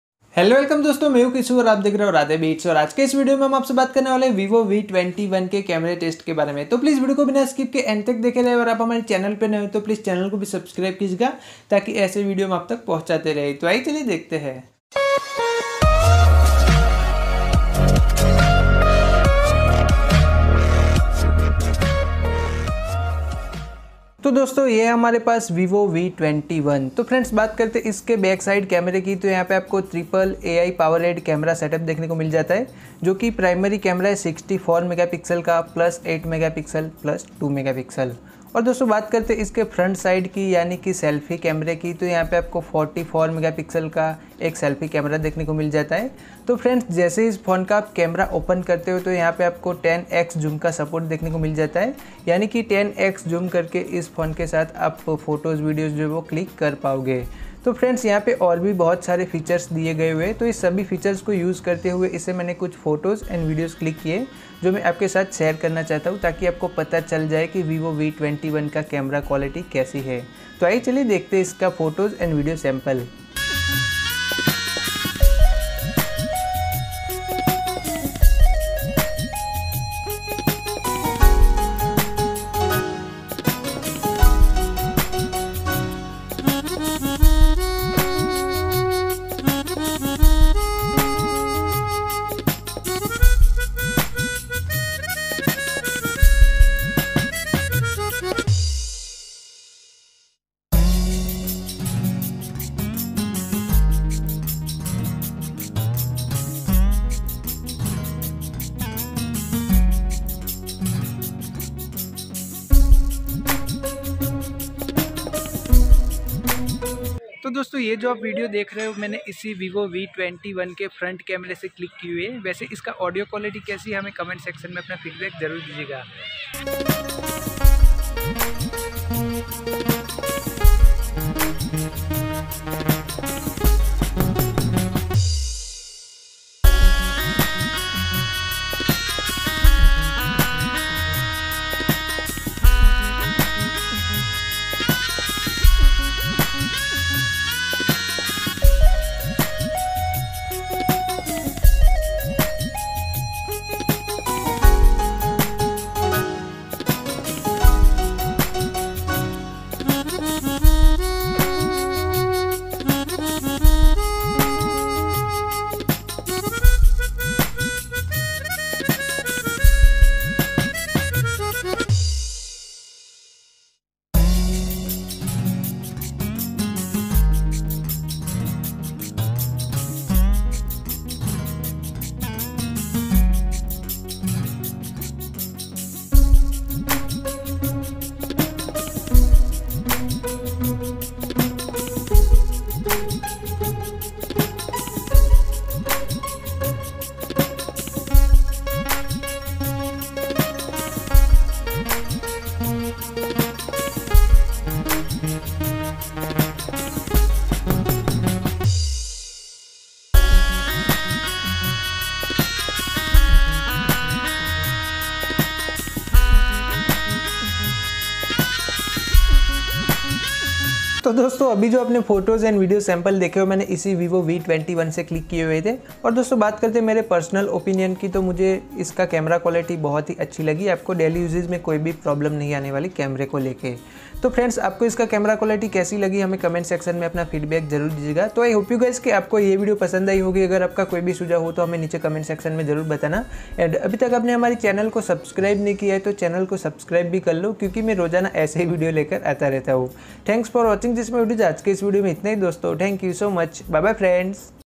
Vivo V21 5G - Camera Test & Features - Photos Semple In Hindi 🔥
In The Box • Handset, Charger, USB Cable, Earphone, Earphone Jack Adapter, Sim Eject Tool, Case, Warranty Card, Quick Guide

• 8 GB RAM | 128 GB ROM

• 16.36 cm (6.44 inch) Full HD+ Display

• 64MP + 8MP + 2MP 44MP Front Camera

• 4000 mAh Lithium-ion Battery

• MediaTek Dimensity 800U Processor

• AMOLED Display

(₹29,990)

‎@Radhe Bits

(Video Upload Time 8:00 AM To 7:00 PM )